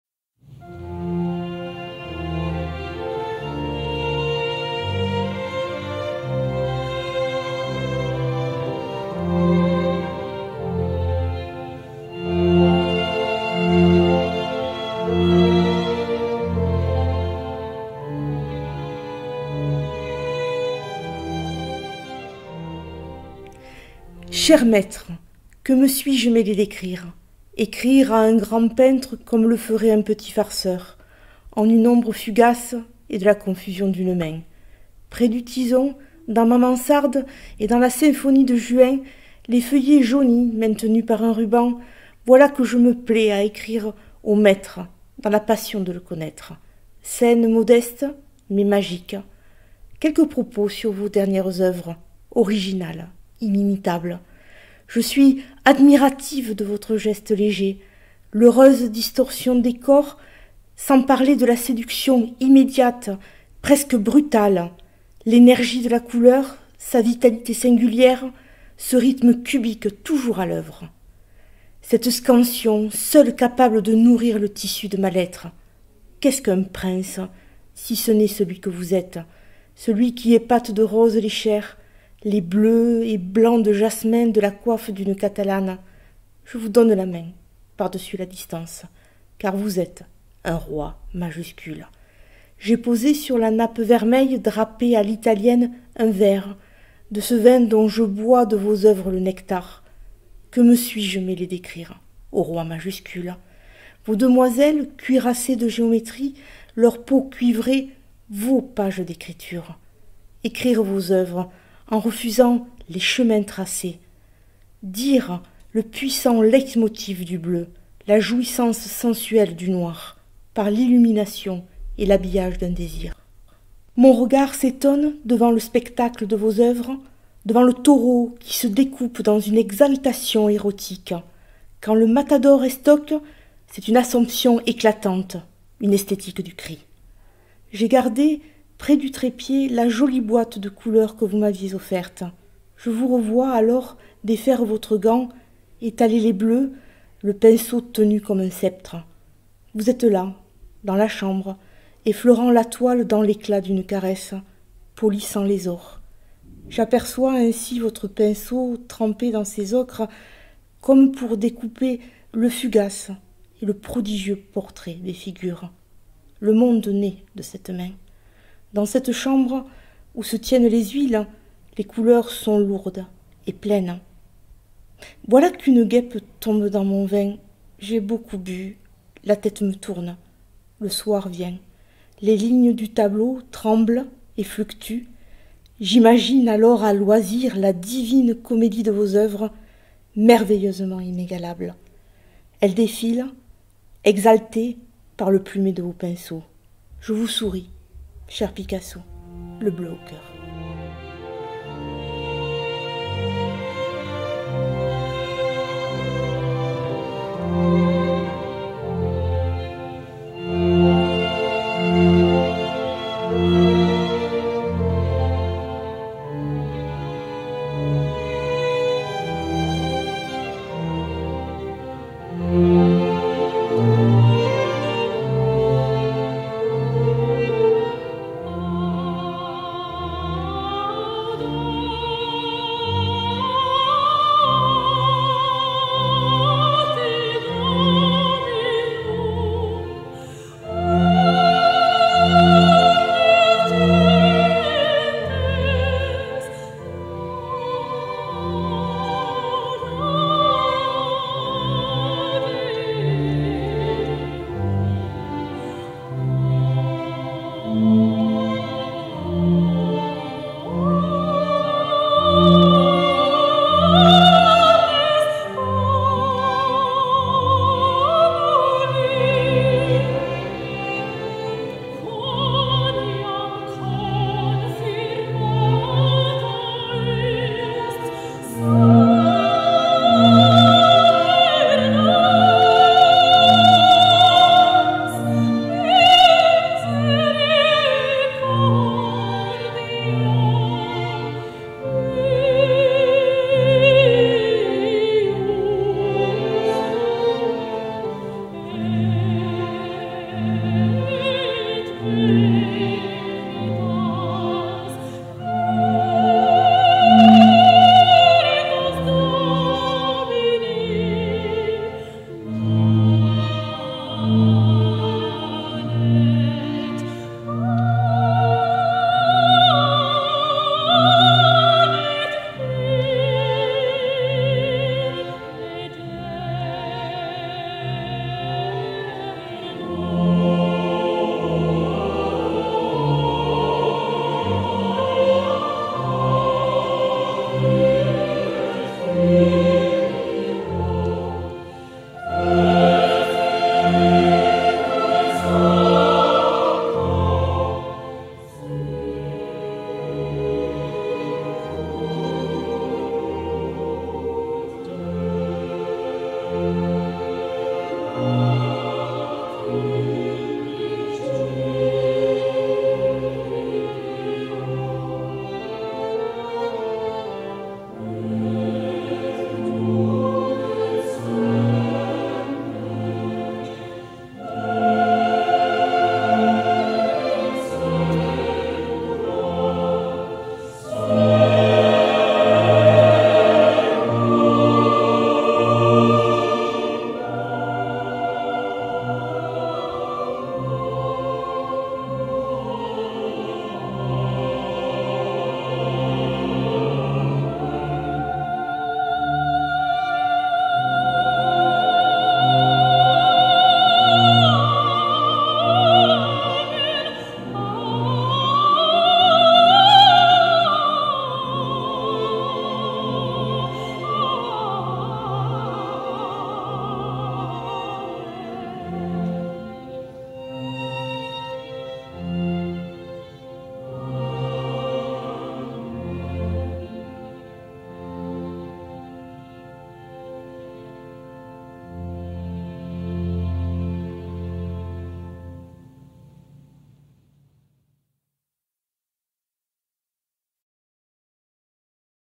[PODCAST] Les Petites Boîtes à Fiction - "Cher Maître", de Stéphanie RAMOS
Ces petites boîtes sont le fruit d'un atelier d'écriture et de création sonore proposé par la médiathèque Arthur Conte de Saint-Laurent de la Salanque. Les participants ont écrit une courte nouvelle, qu'ils ont enregistrée et agrémentée d'une ambiance sonore avant de la diffuser sur la chaîne Youtube du réseau des bibliothèques de Perpignan Méditerranée Métropole. Chaque texte tient dans une petite boîte en carton et comprend un QR-Code qui renvoie à sa version audio. Le thème de l'édition 2023 était : En toutes lettres.